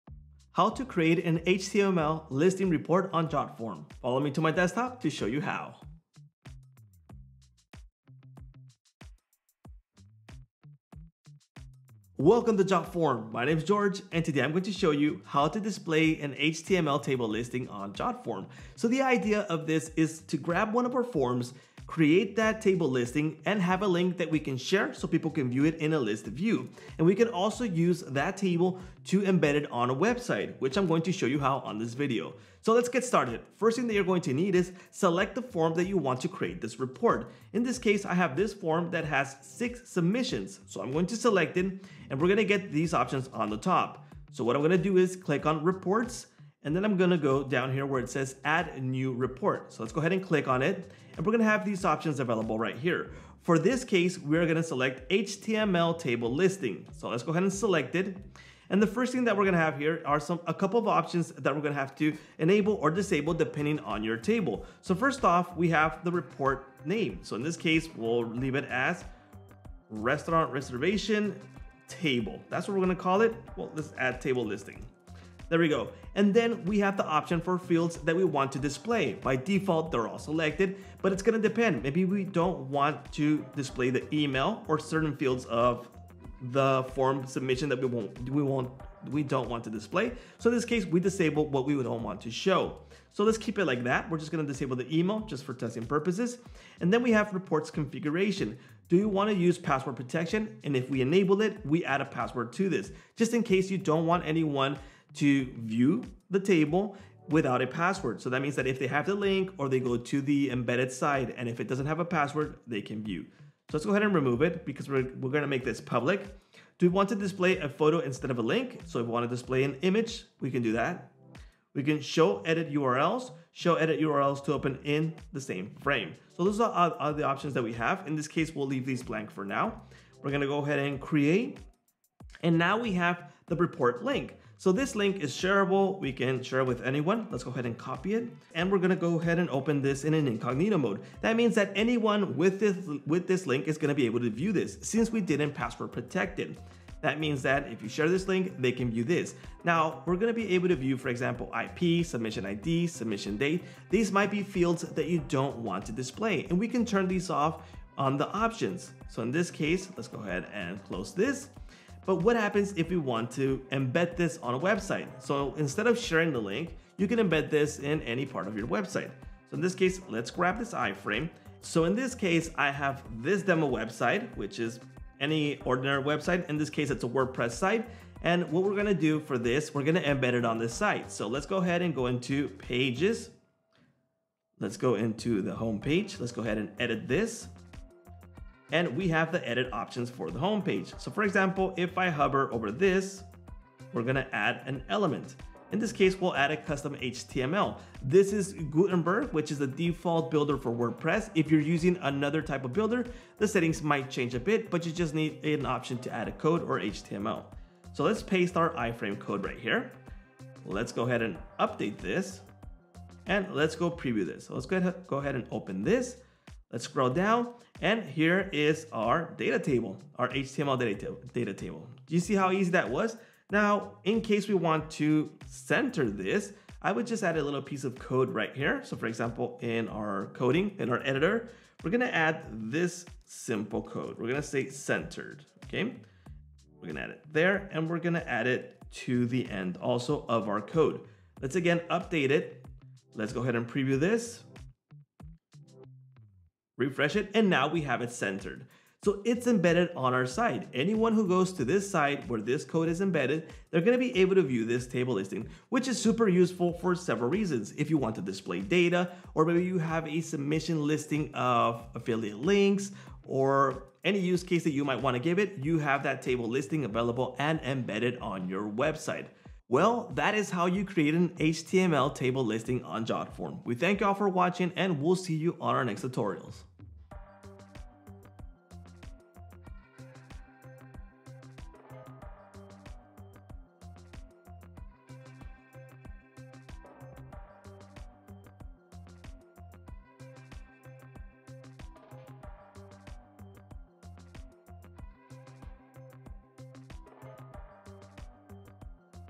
How to Create an HTML Table Listing Report
Did you know you can easily create HTML table listings on Jotform? This is great if you want to share submission reports with others in a list view, either using a copy-paste report link or by embedding it in a website. Learn more in this brief video tutorial from Jotform.
LINKS & RESOURCES
👋 ABOUT JOTFORM
Hi, we're Jotform, a full-featured online forms platform that makes it easy to create robust forms and collect important data. Check us out
00:00 Introduction
00:12 Overview
00:34 Select the Form That You Want to Create a Report From
00:49 Create a New Report
00:58 Select HTML Table Listing
02:11 Copy Report Link
02:30 View the Data Table in Incognito
02:50 Copy Iframe Code
03:01 Embed on the Site
03:14 Edit Home Page
03:34 Preview in a New Tab
03:50 Enter a Code to the HTML to Center the Data
04:36 Subscribe to Jotform